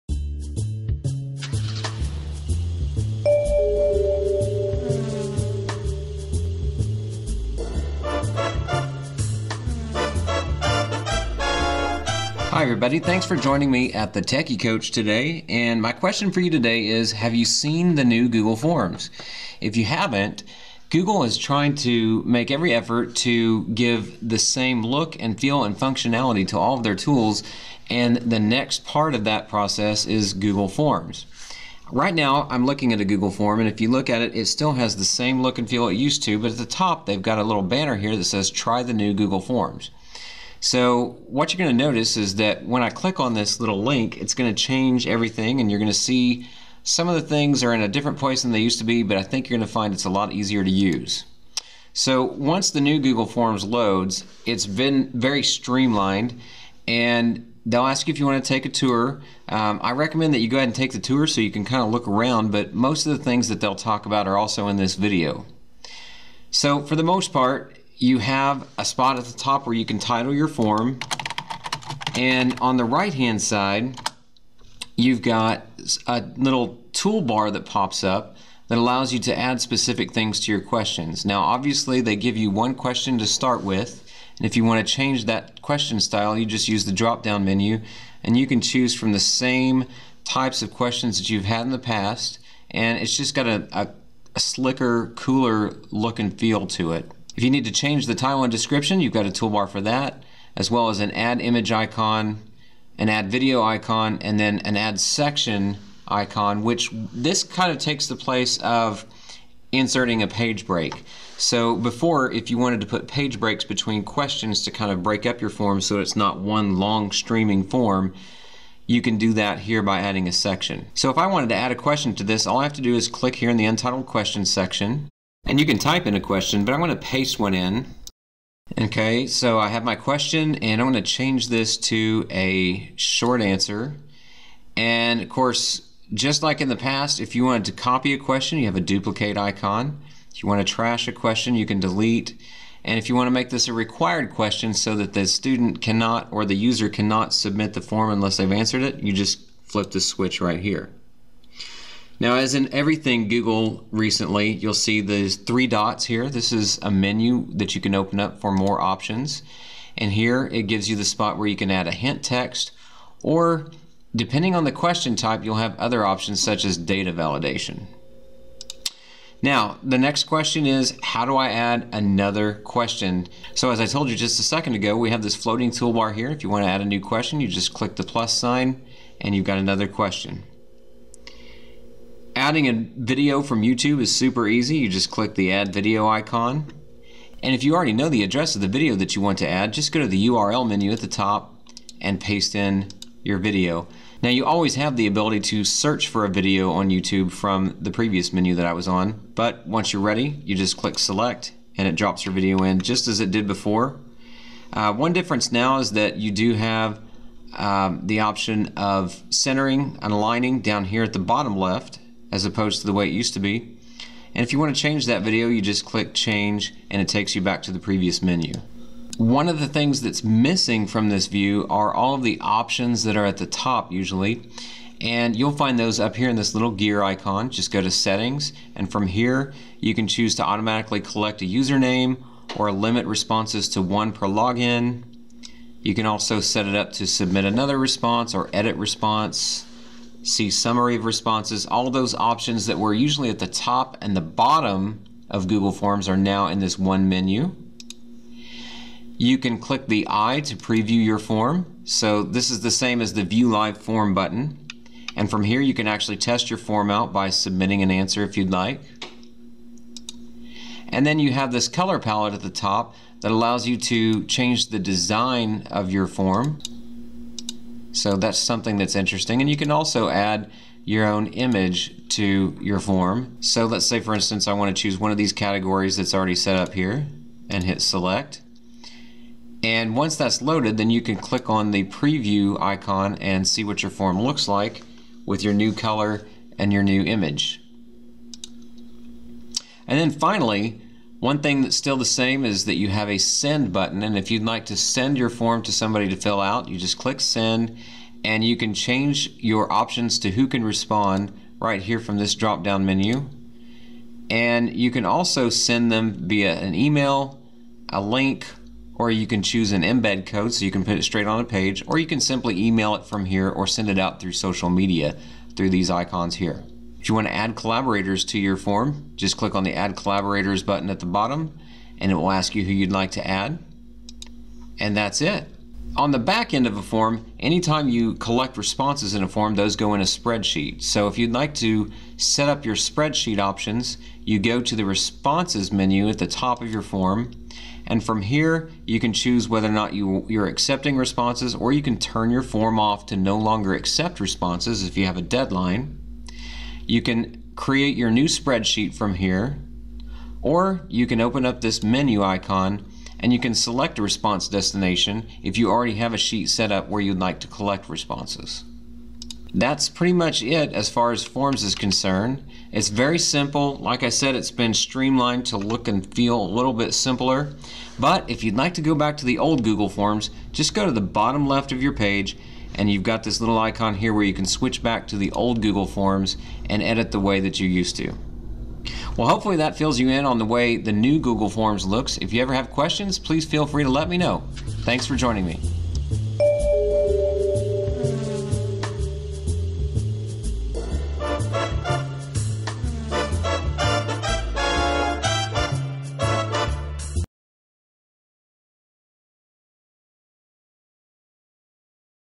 A Quick Look at the New Google Forms
This video takes a quick look at the new look and feel of Google Forms